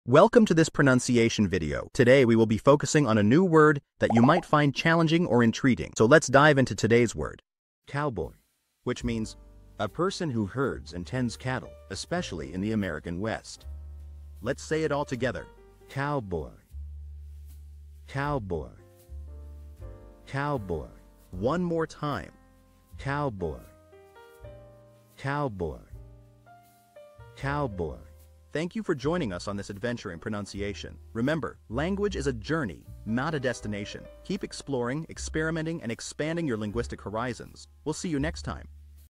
How to pronounce Cowboy
Master the Pronunciation of 'Cowboy' - which means : A person who herds and tends cattle, especially in the American West. 📝 with @PPrononciation 🎓🗣️ - [Your Guide to English]

Learn to pronounce 'Cowboy' perfectly in English with our easy and engaging tutorial by @PPrononciation! 📖📽️ In this comprehensive video, we provide you with native speaker examples 🗣️👂 and phonetic breakdowns 🎵 to ensure you get the pronunciation just right.

How to say Cowboy in English